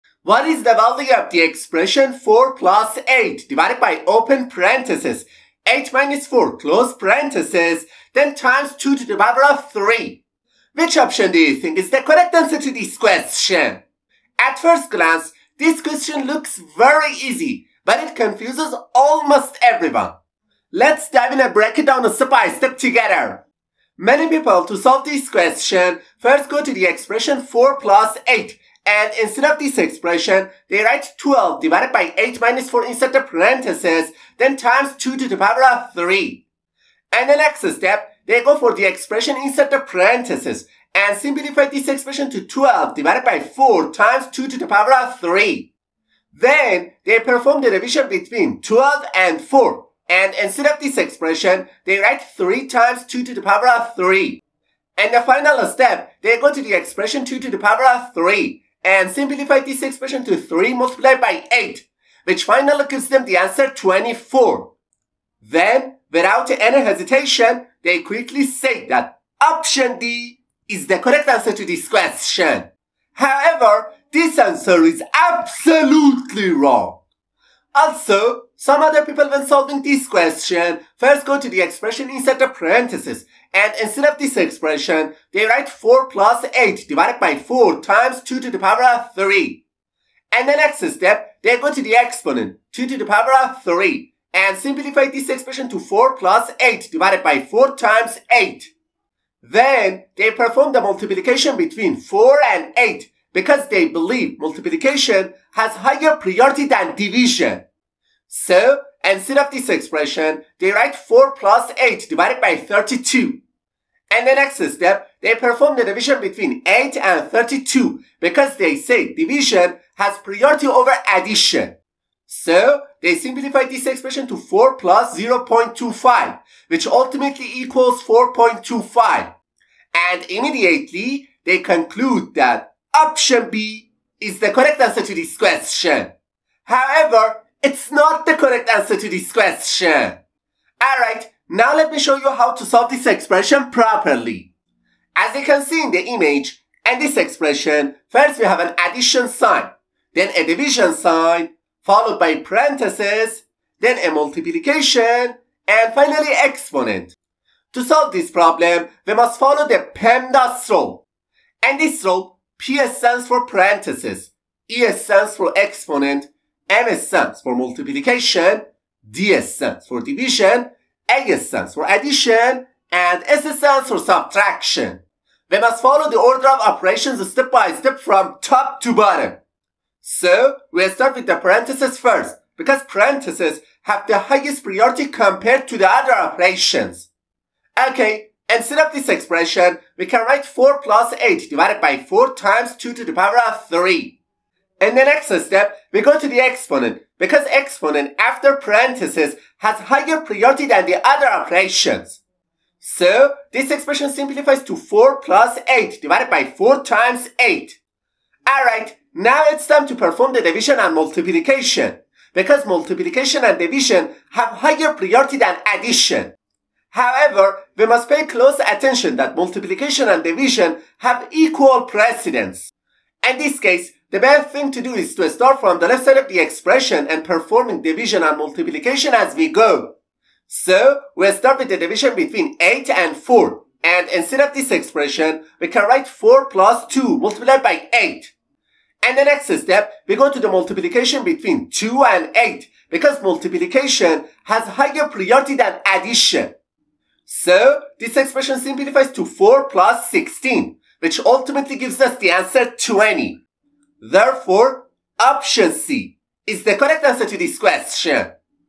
Can You Solve This Tricky Math Question Without Making a Mistake? 99% Get It WRONG! 🤯
🧠 Think you know PEMDAS? Think again! This math question looks super easy… but almost everyone gets it WRONG! 😱
Can you follow the correct order of operations and find the right answer?

In this video, we’ll break down a tricky expression step by step and see who’s really a math genius. 💪
Don’t rush—one small mistake can completely change the answer!

🔹 Pause the video and try it yourself.
🔹 Comment your answer below and see if you’re smarter than 99% of people! 😎